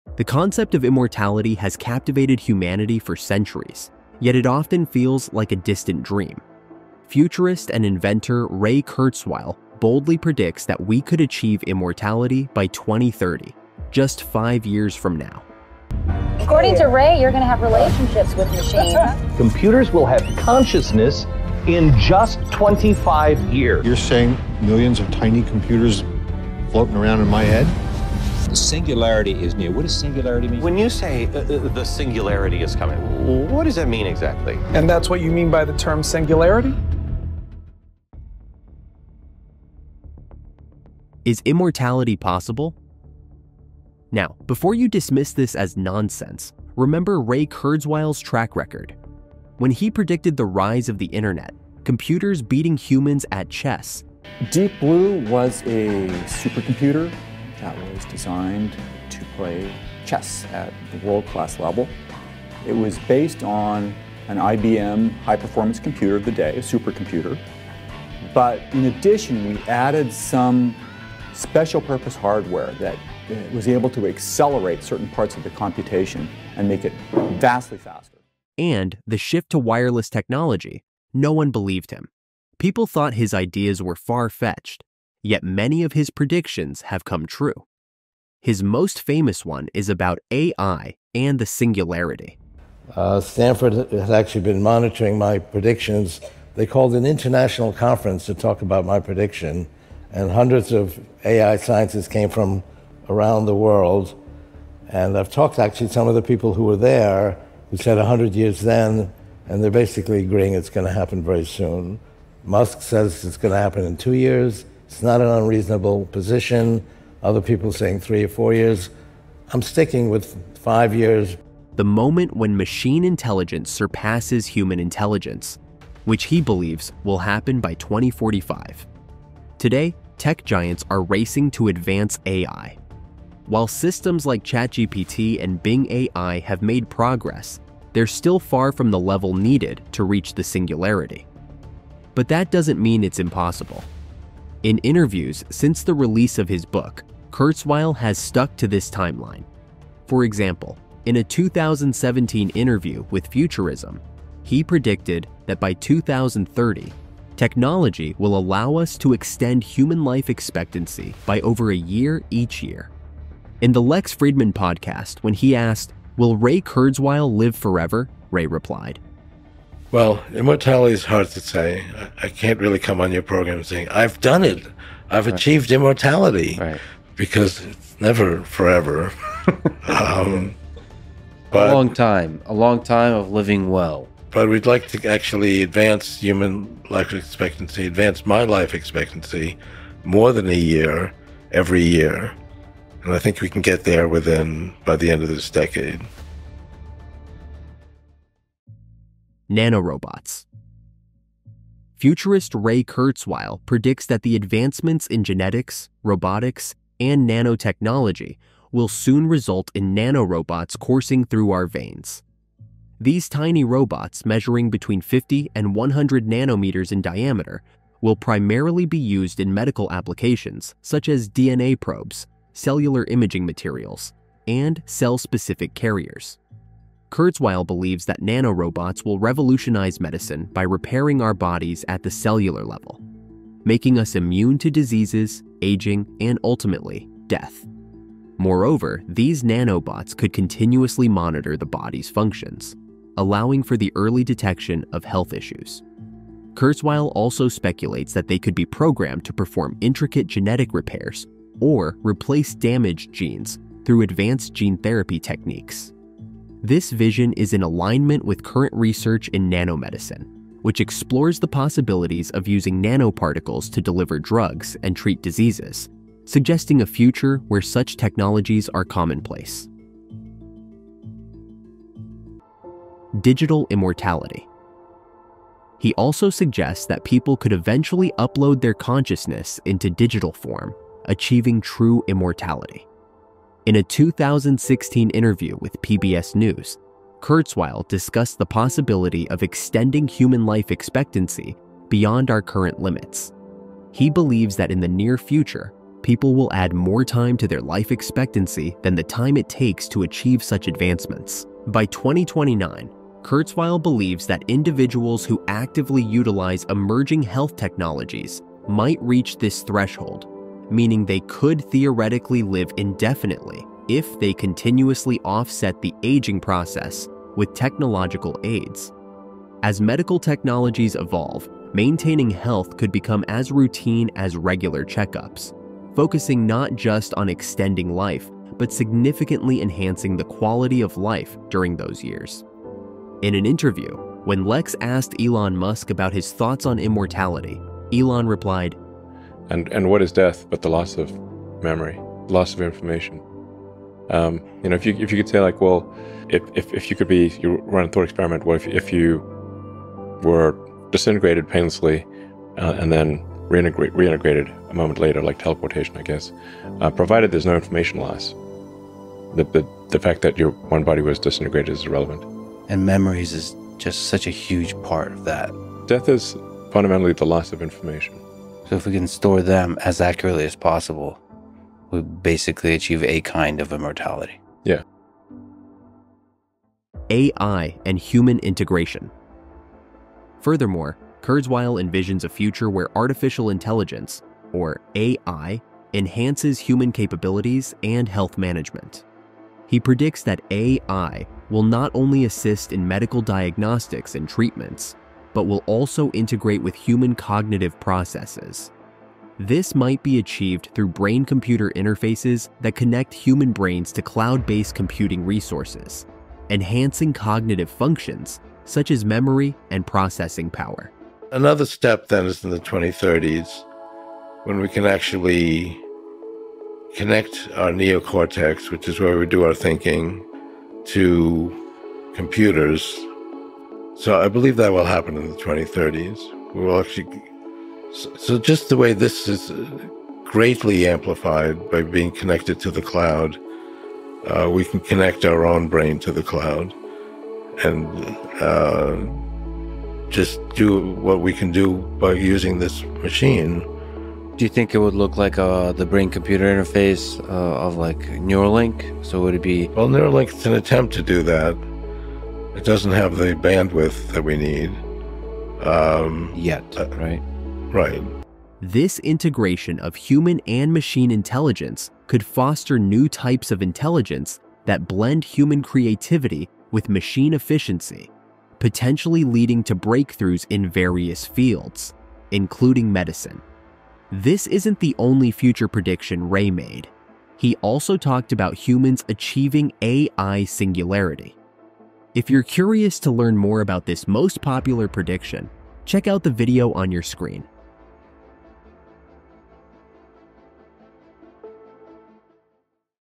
Ray Kurzweil: Does AI Make Immortality Possible?
In this video, we look at Ray Kurzweil's bold claim that AI could make immortality possible by 2030. We’ll explore how nanotechnology, digital consciousness, and blending AI with human biology could make living forever a real option. From tiny robots that could repair our bodies to the idea of uploading our minds into a digital world, Kurzweil’s vision shows a future that’s both amazing and a little scary.

Join us as we talk about what immortality might look like, the possible risks, and how AI and humans could merge in ways we’ve only seen in science fiction.

Chapters:
Intro 0:00 - 0:39
Is Immortality Possible? 0:40 - 3:19
Nanorobots 3:20 - 4:31
Digital Immortality 4:32 - 6:16
AI and Human Integration 6:17 - 8:12